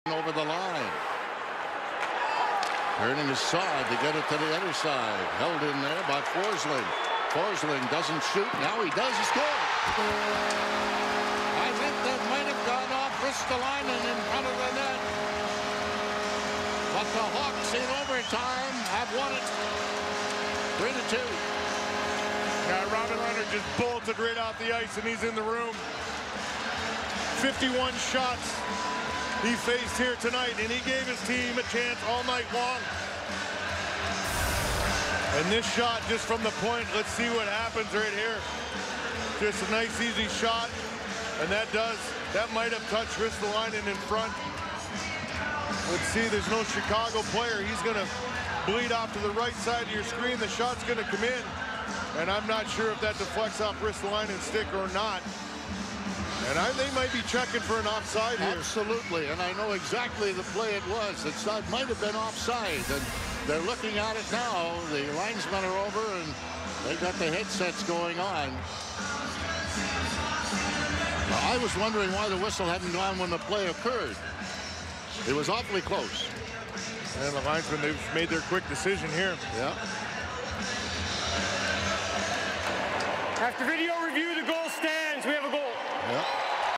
Gotta See It: Blackhawks' Forsling walks the line and wins it in OT
Chicago Blackhawks' Gustav Forsling shows some nice poise on the back end, walking the blue line and sneaking a wrist shot under the pad of Robin Lehner with only five seconds remaining.